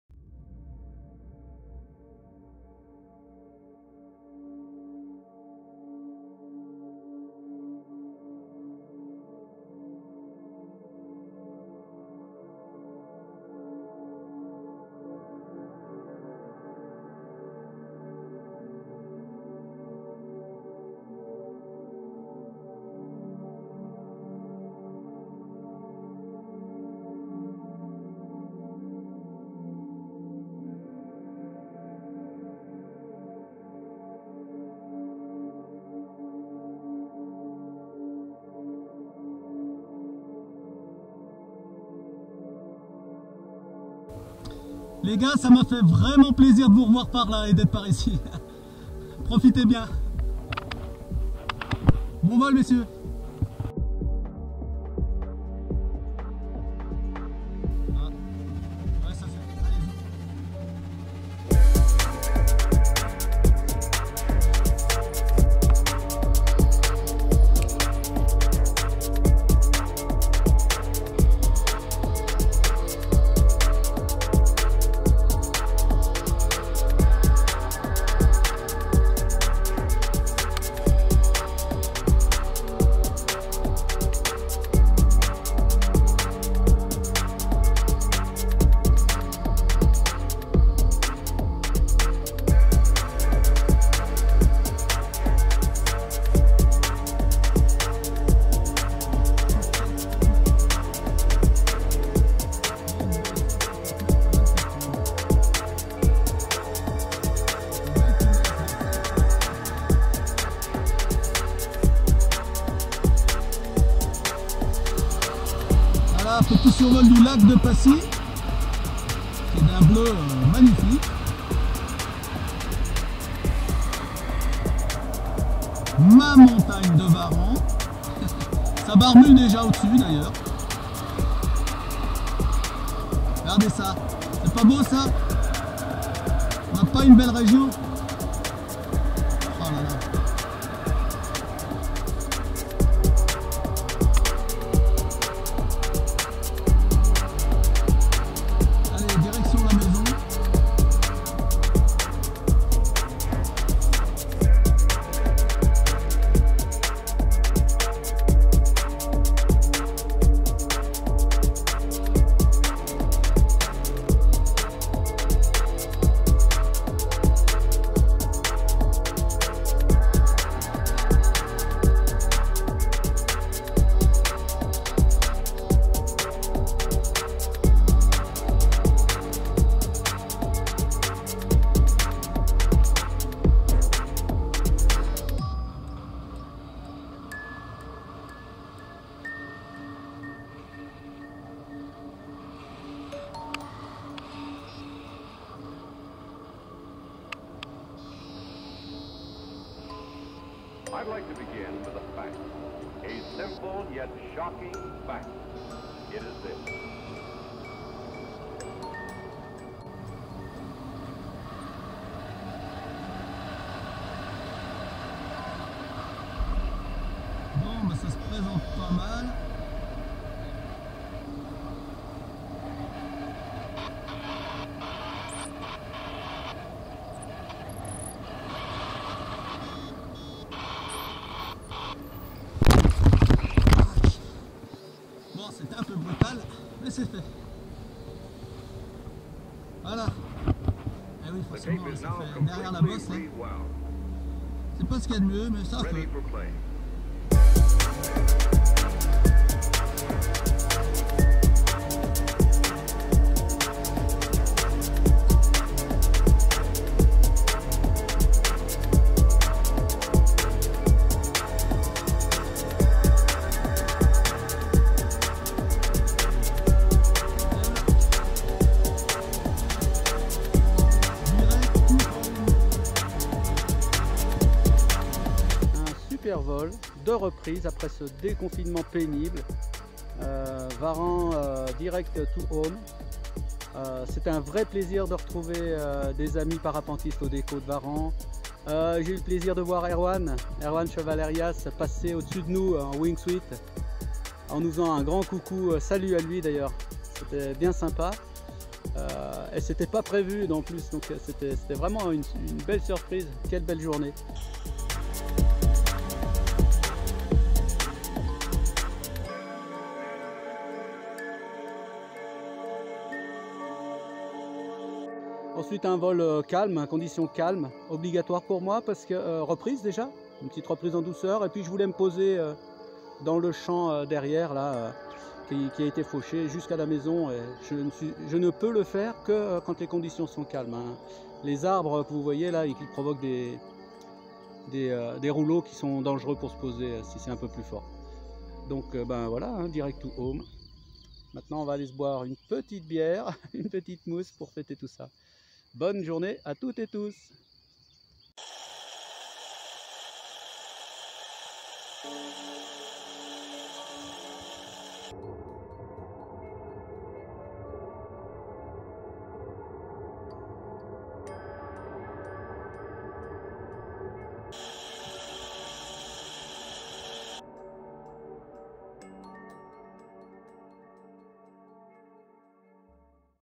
Déconfinement Où ça ? Varan évidemment .Avec une belle Surprise.
Quel plaisir, après ce confinement et ce Covid19 qui ne m'avait pas épargné. Me laissant les poumons affaiblis, j'ai galéré pour marcher jusqu'au déco de varan. Mais cela en valait largement la peine. Quel bonheur de voir passer mon ami Wingsuiter , Erwan. Sans que cela ne soit prévu, j'ai reconnu sa combinaison. C'était énorme ! de l'entendre et le voir fendre l'air de ma montagne préférée. Vraiment , après ce fléau, ce virus qui pour trop d'entre nous à causé la perte d'être cher, pouvoir ressortir et voir ces amis, même très brièvement ;-D (Erwan !) fut une bouffée d'oxygène essentielle , venue au bon moment. Bon visionnage les amis, et concerver les regles sanitaires. Prenez soins de vous !